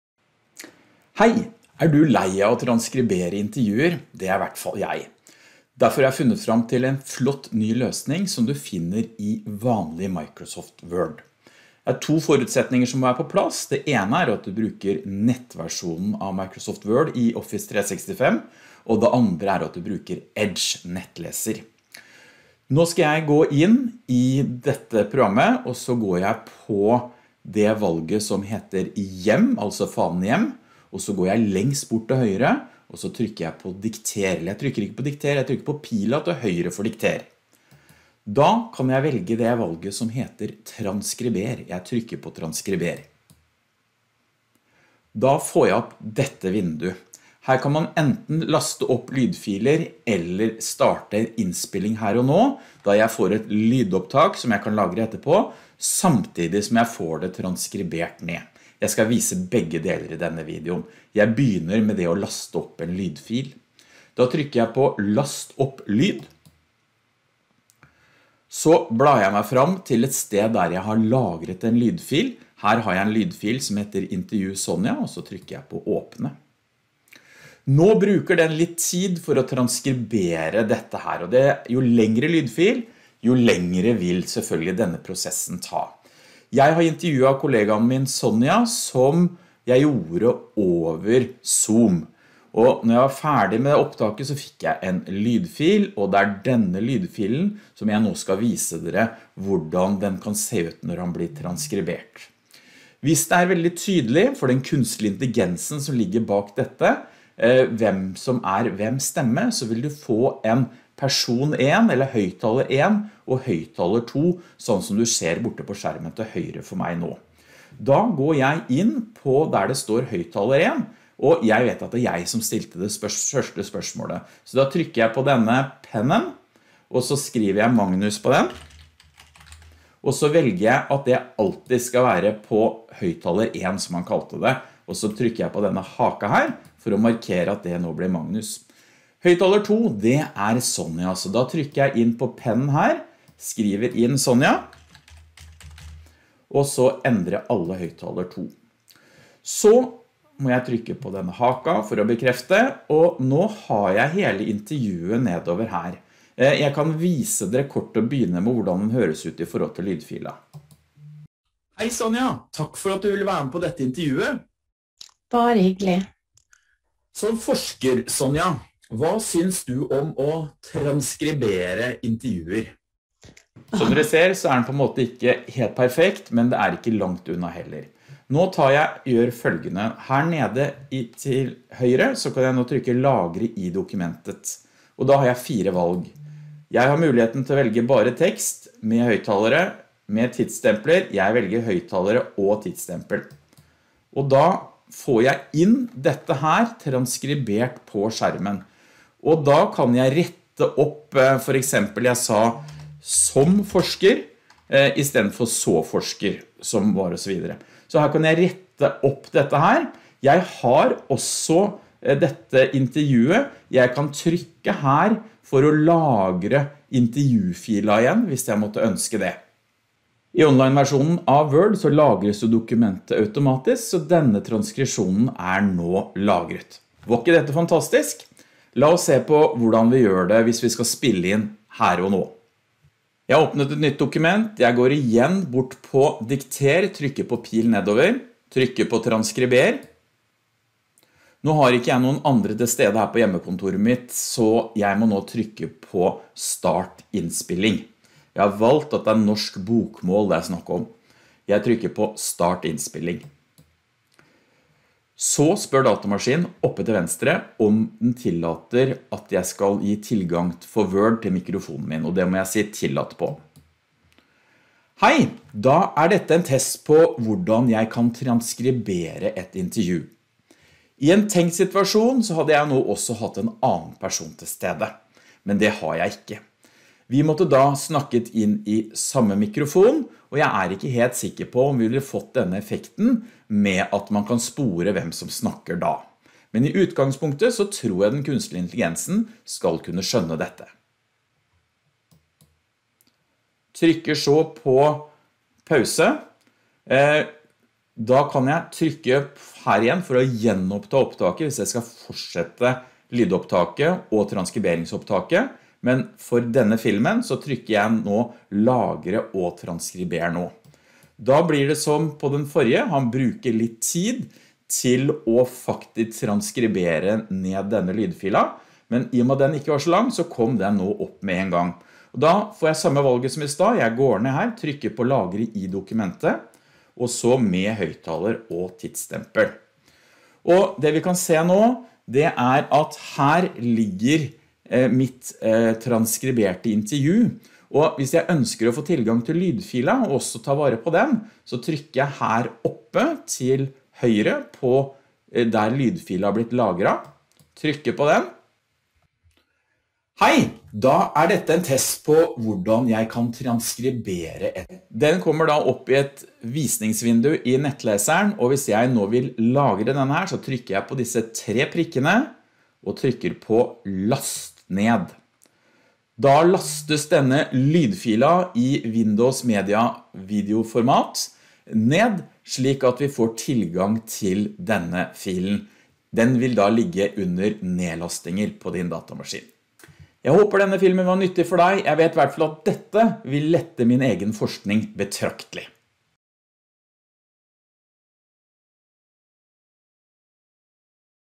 Automatisk transkribering av lydfiler i Word online
I denne videoen viser jeg hvordan du kan få Automatisk transkribering av lydfiler i Word online i Office 365.